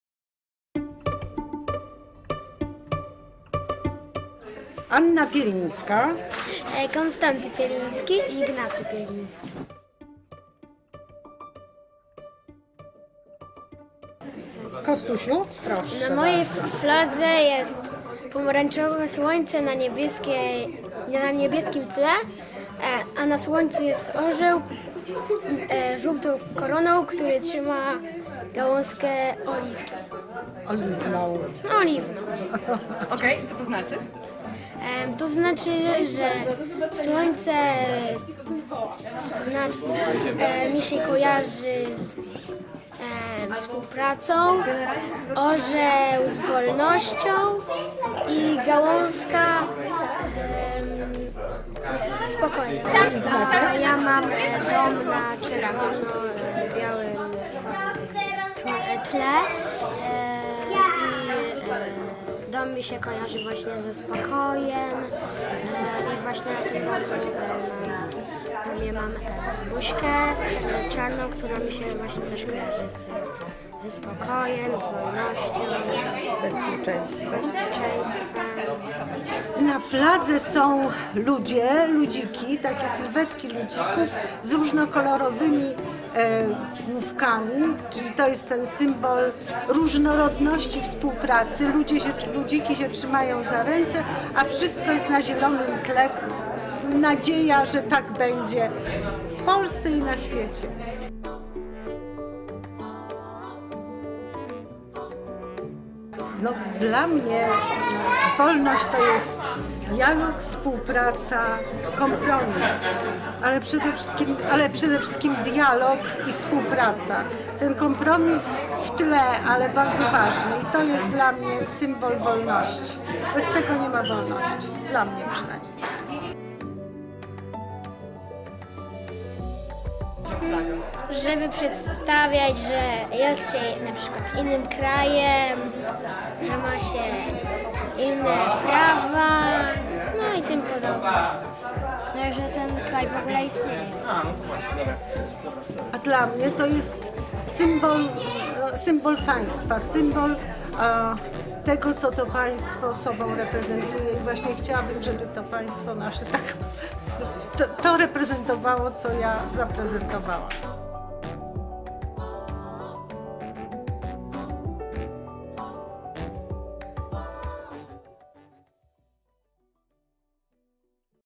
101 flag na 101 lat / prezentacja kilku autorek i autorów flag / 1
Finał projektu "101 flag na 101 lat" już 12 listopada, o godz. 18.00. Zaczynamy prezentację kilku autorów i autorek flag, które pojawią się na Palcu Defilad. W pierwszym odcinku babcia Ania wraz wnukami - Ignacym i Kostkiem czyli międzypokoleniowy zawrót głowy w słusznej sprawie. Oglądajcie i słuchajcie uważnie!